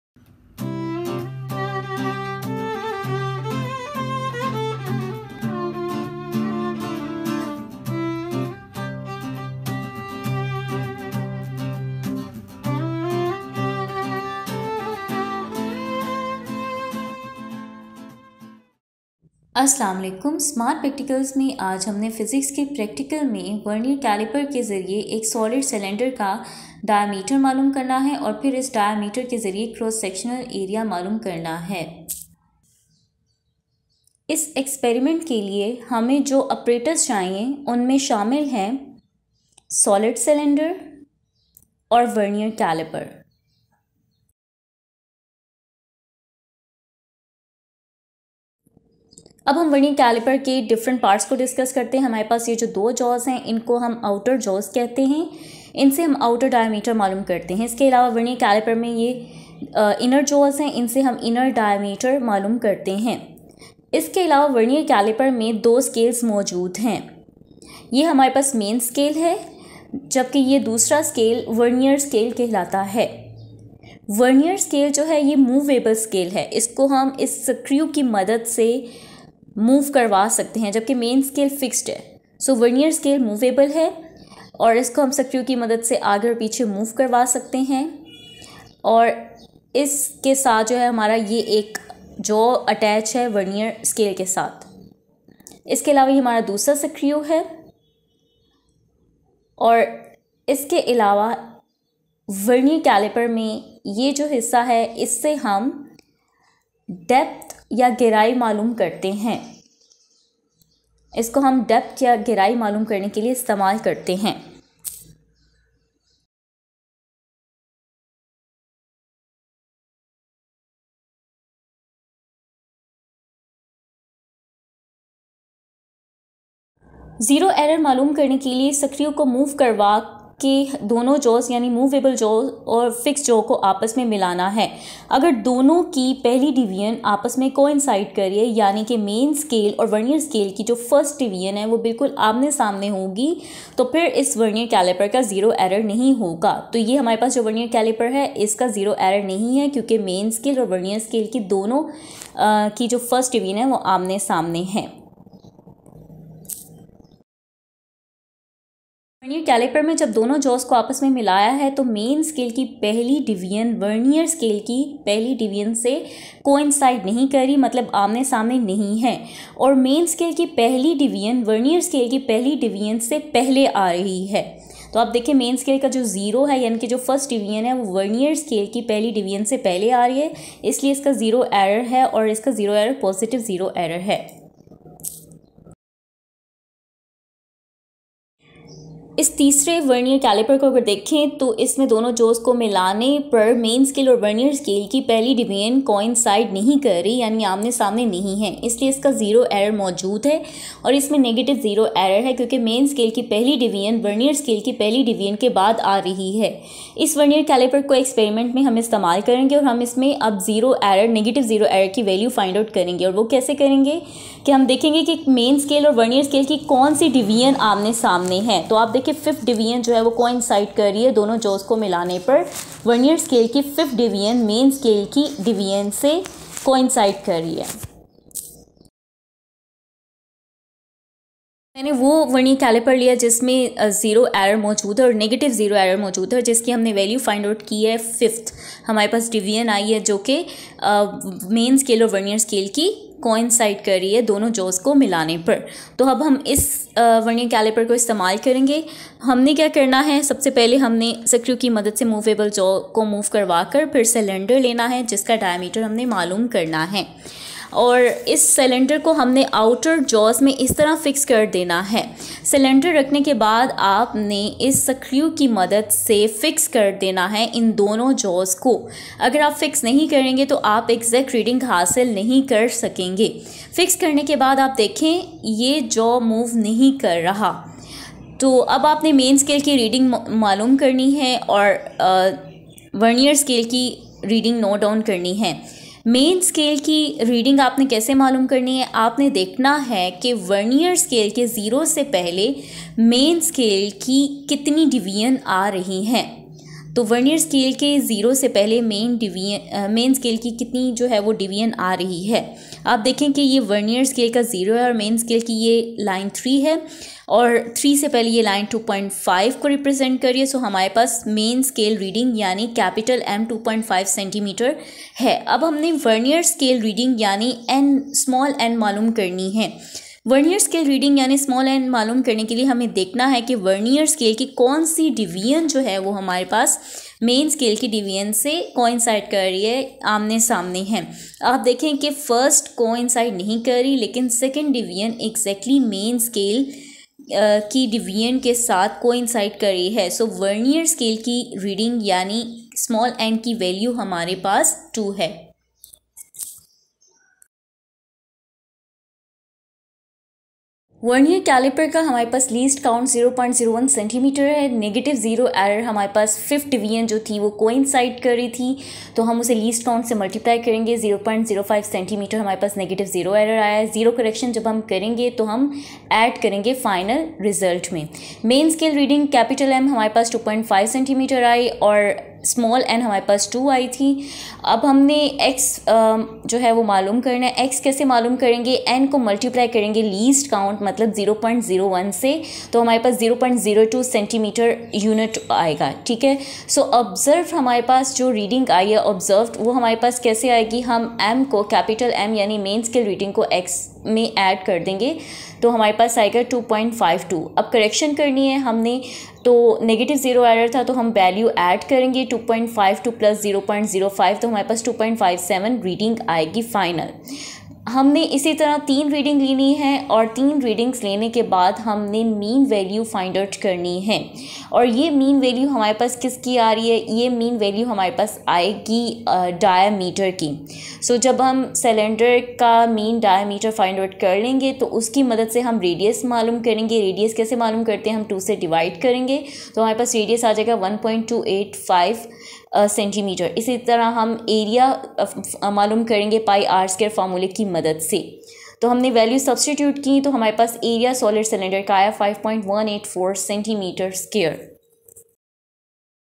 Experiment 1: Measure the area of cross-section of a solid cylinder by measuring its diameter with..
Measure the area of cross-section of a solid cylinder by measuring its diameter with Vernier calipers.